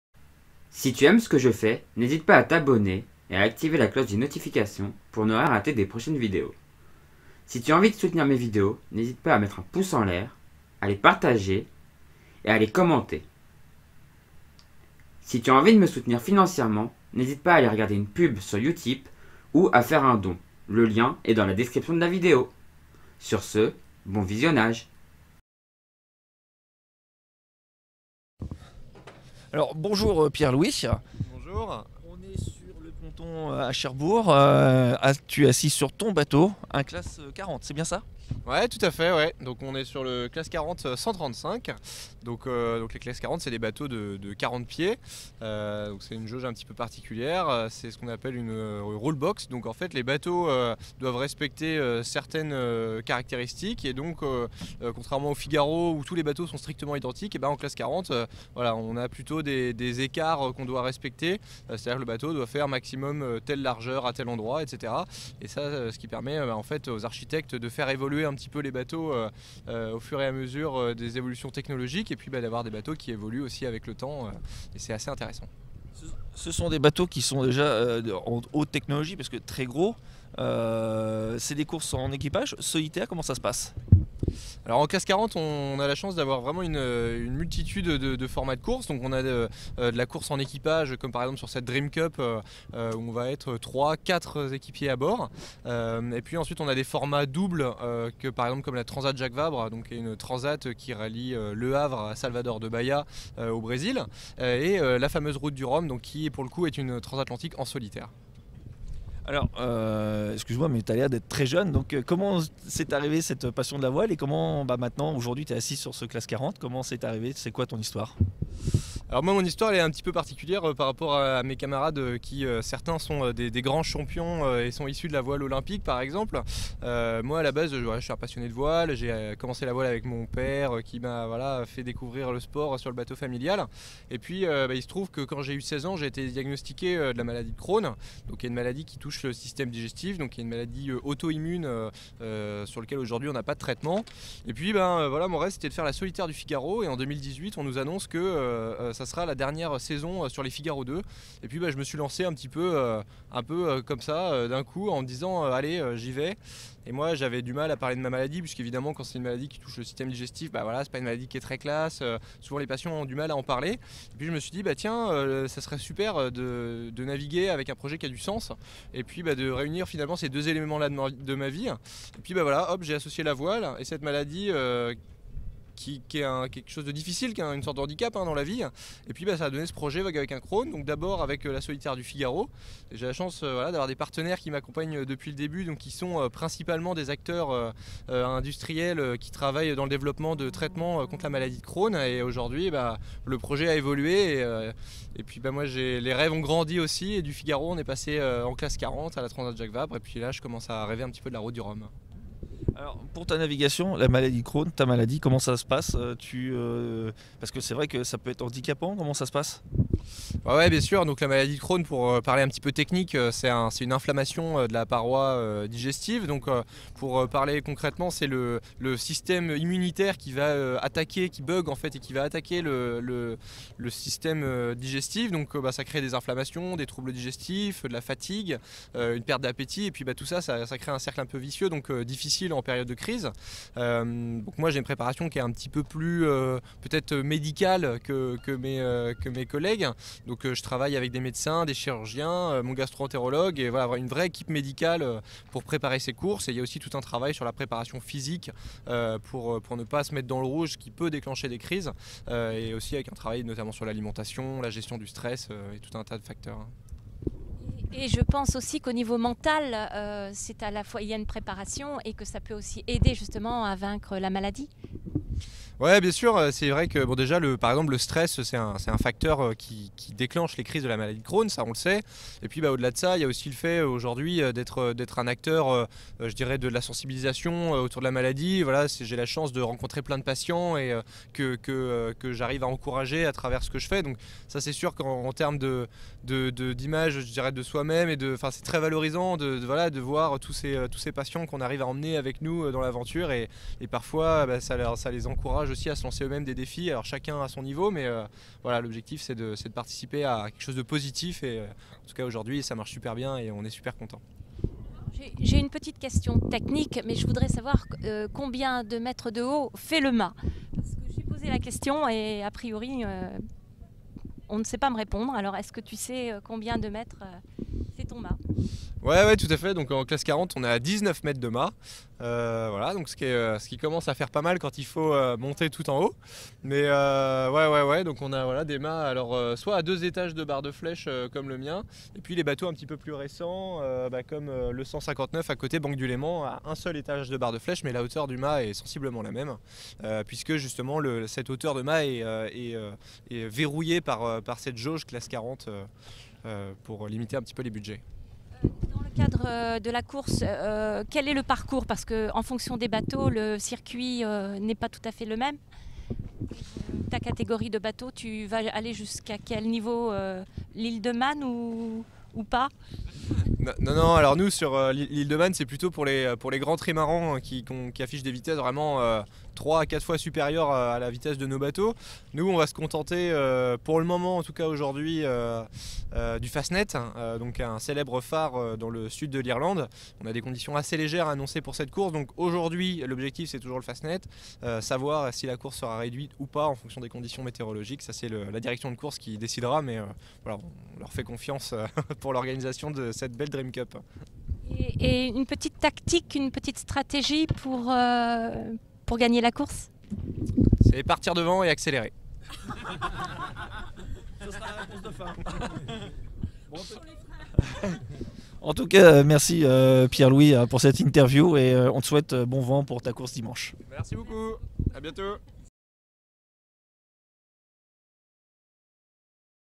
Les voix de la Drheam cup: Pierre Louis Attwell un skipper courageux
Dans cette série de vidéo ont interview une personnalité ou un marin de la Drheam cup.

twiter: @toria_et